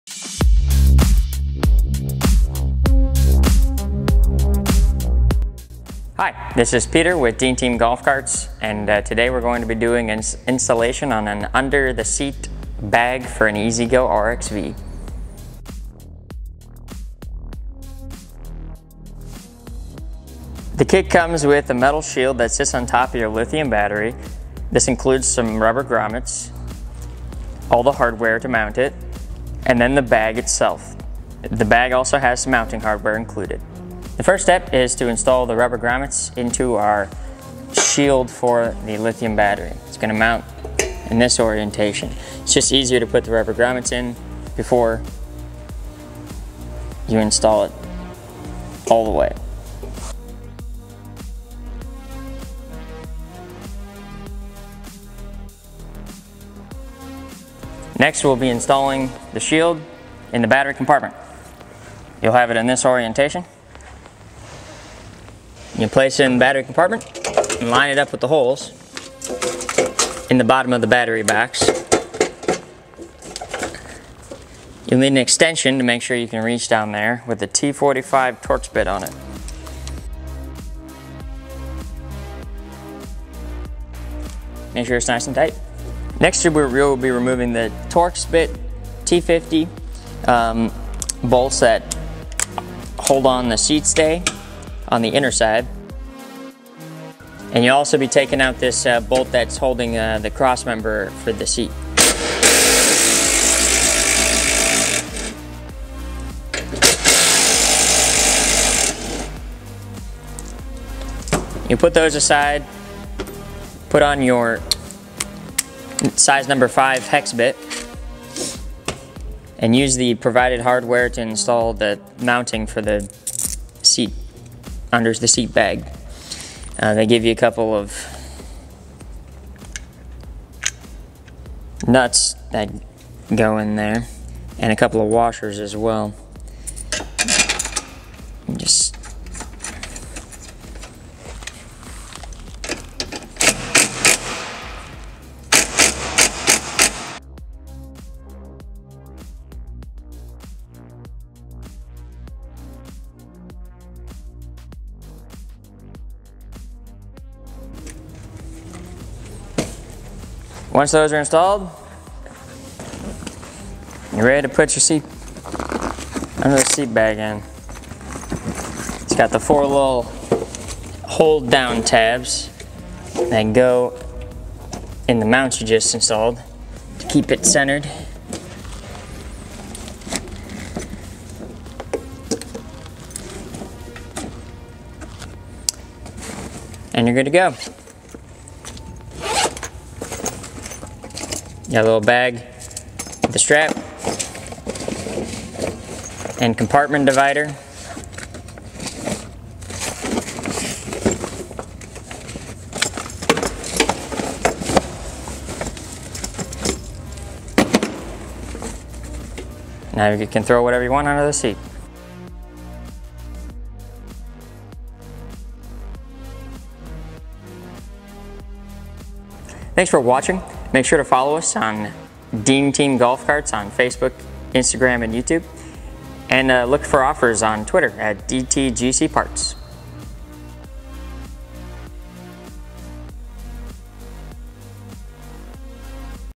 EZGO Storage Bag Installation | Dean Team Golf Carts
Upgrade the storage capacity on your golf cart with this discreet under the seat storage bag from EZGO! Peter from our service department is here today to show you how you can install one.